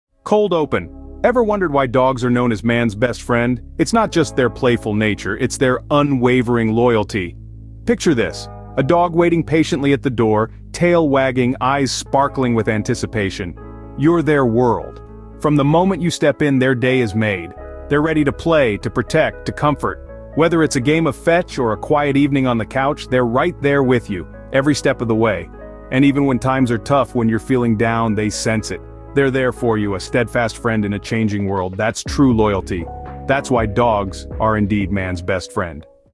Loyal Paws: A Tribute to Man's Best Friend
Join us in this heartwarming and entertaining video as we celebrate the unwavering loyalty of our furry friends to their human companions. Prepare to be amazed and touched by the incredible bond between dogs and their owners, demonstrated through heartwarming moments and captivating stories. Set to a fun and upbeat soundtrack, this video will leave you feeling inspired and uplifted. Let's appreciate the extraordinary loyalty of dogs together.